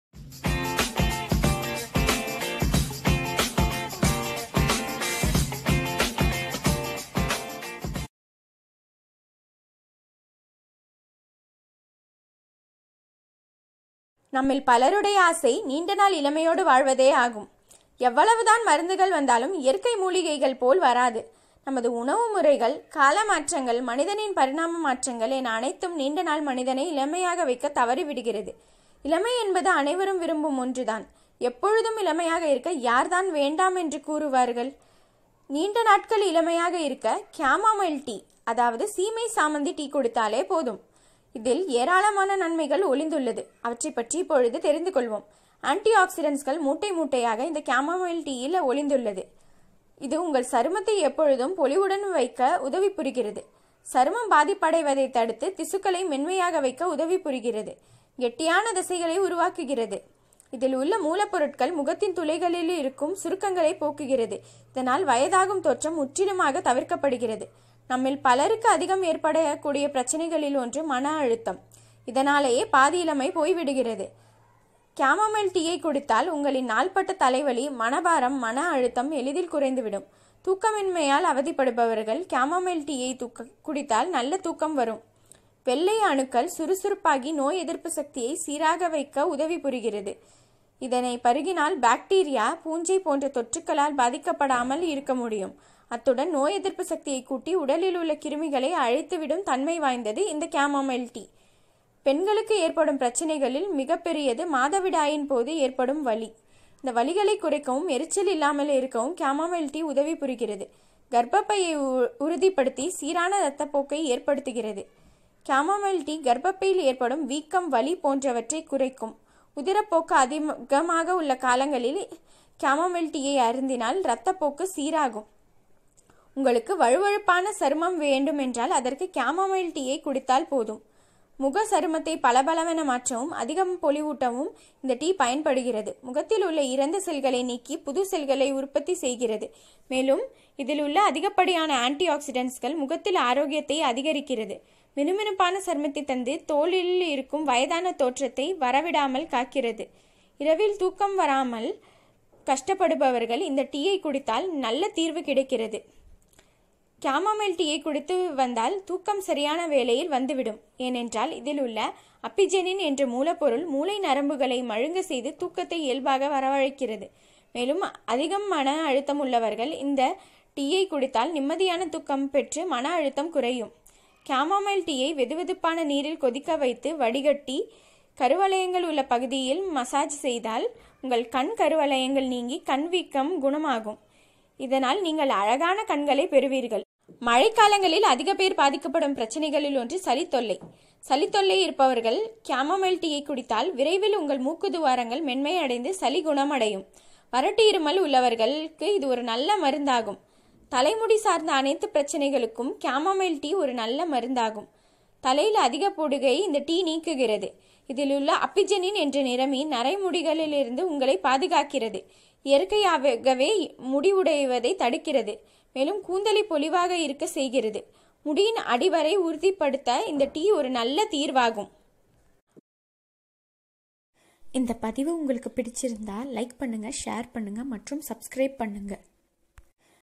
கெமொமில் டீயின் நன்மைகள் | இளமையோடு வாழ சீமை சாமந்தி தேநீர் | Benefits of chamomile tea
இளமை என்பது அனைவரும் விரும்பும் ஒன்றே. எப்போதுமே இளமையாக இருக்க, யார்தான் வேண்டாம் என்று சொல்லவர்கள். இப்படி தீரா மோகம் இளமை மீது நாம் கொண்டிருக்க அதற்கு தீர்வு இல்லாமலா போய்விடும். ஆமாங்க, நீண்ட நாட்கள் இளமையாக இருக்க இந்த சீமை சாமந்தி டீ (Chamomile tea) குடித்தாலே போதும். மேலும் இதில் ஏராளமான நன்மைகள் உள்ளது. அவற்றை பற்றி இந்த பதிவில் பார்க்கலாம்.